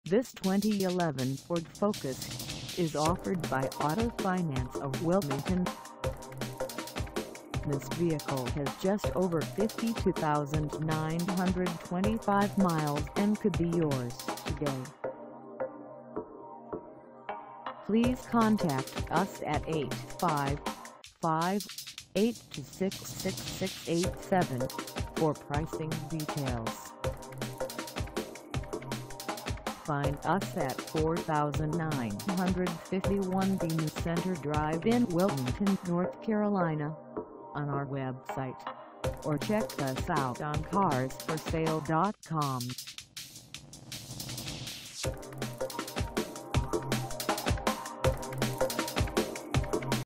2011 Ford Focus SE 4dr Sedan for sale in Wilmington, NC 2840
This 2011 Ford Focus SE 4dr Sedan is for sale in Wilmington, NC 28403 at Auto Finance of Wilmington.
Engine: I4 2.0L I4
Transmission:
Color: White Suede
Mileage: 52925
Contact Auto Finance of Wilmington 4951-B New Centre Dr. Wilmington, NC 28403 to test drive this 2011 Ford Focus SE 4dr Sedan today.